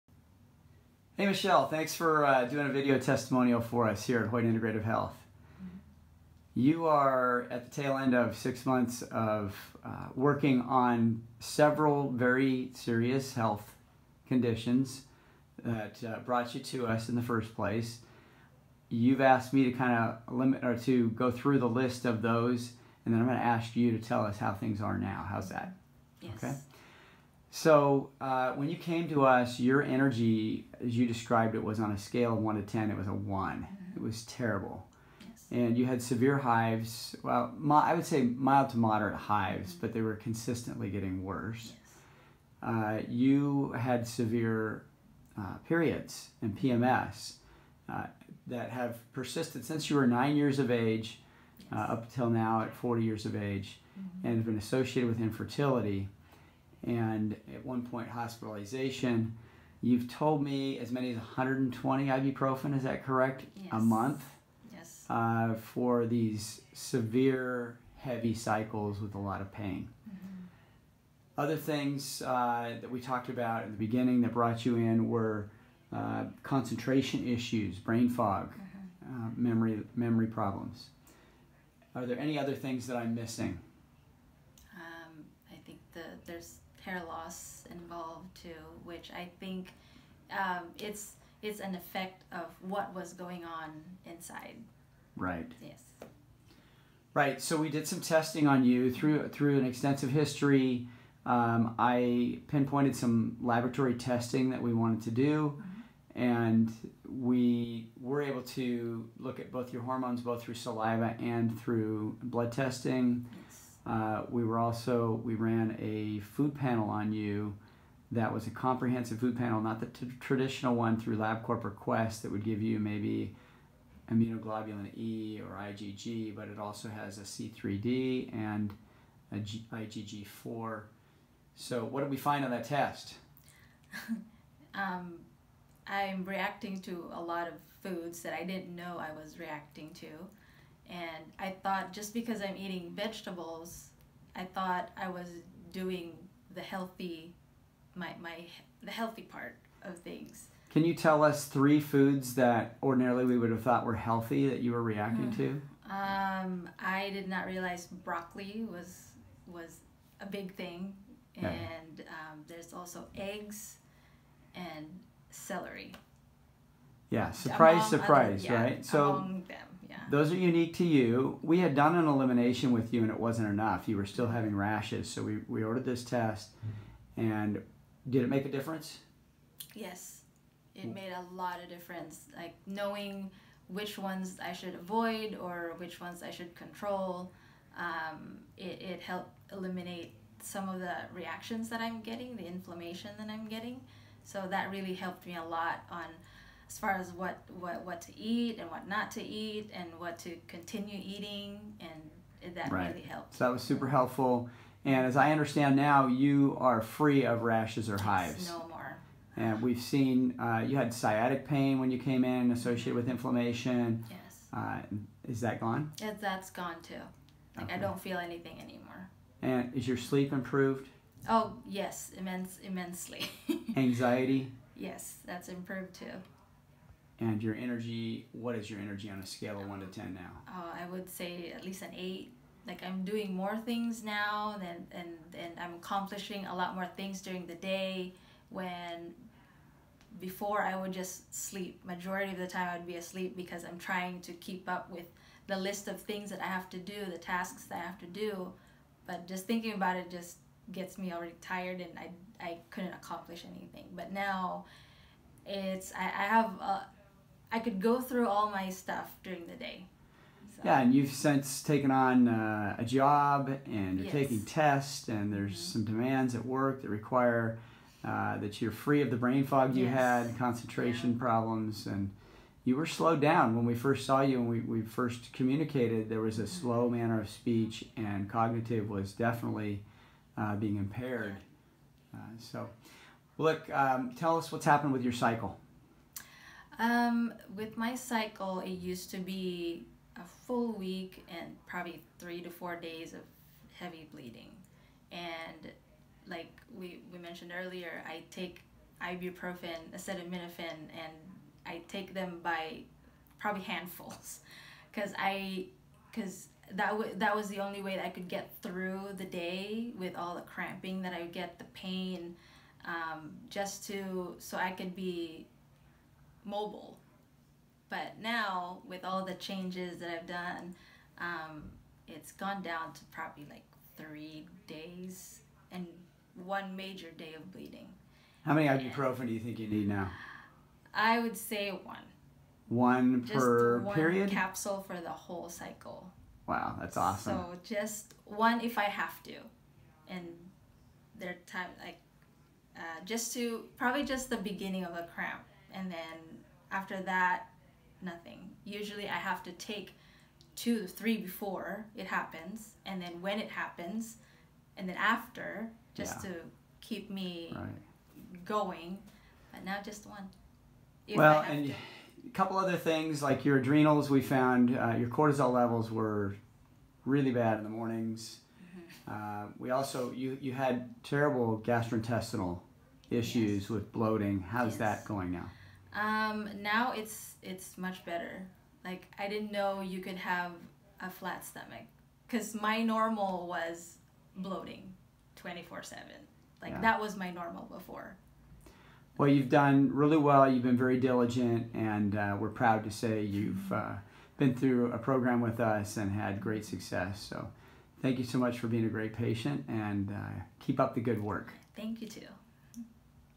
Michelle-Severe Periods Since Childhood, Brain Fog, Fatigue, Depression, Insomnia, Infertility
Fatigue, Depression, Insomnia, Hives, Heavy Painful Cycles, Brain fog